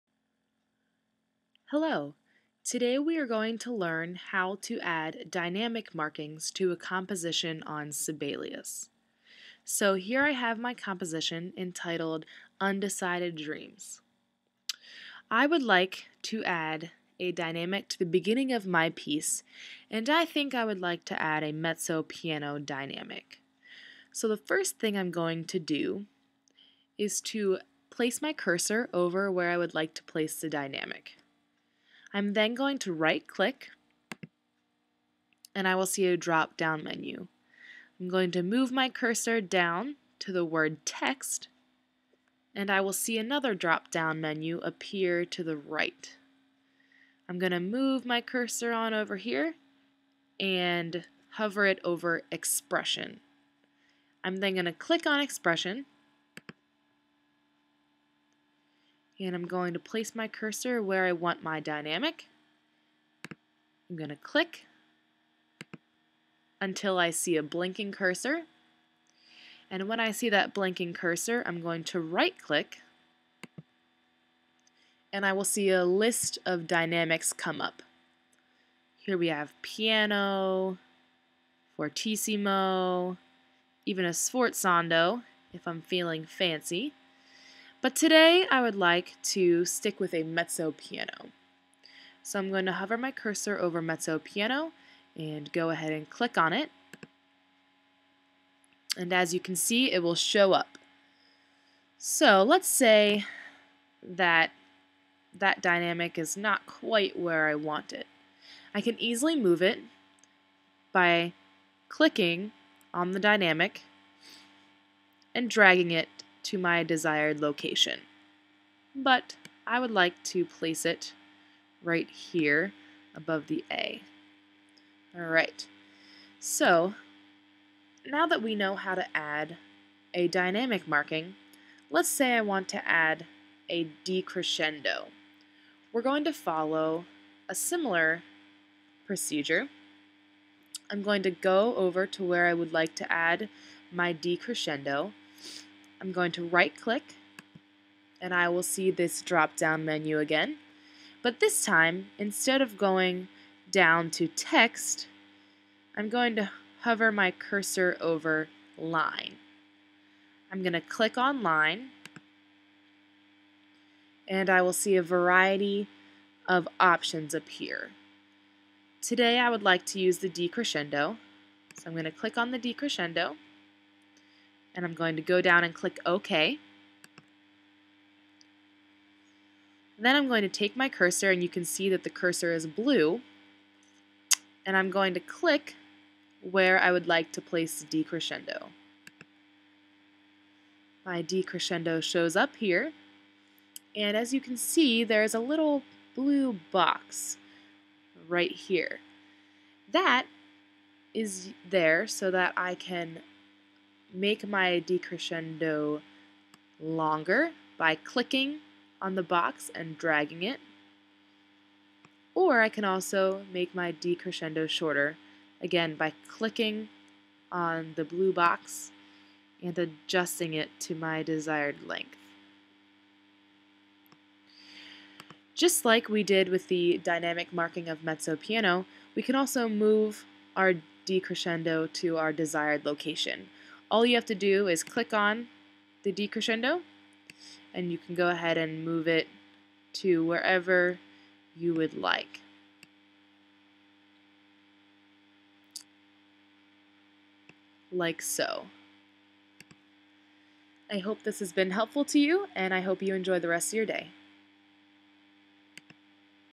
How to add dynamics on Sibelius Music Notation Software
This brief video demonstrates how to add dynamic markings such as mezzo-piano (mp) and decrescendo to a Sibelius composition.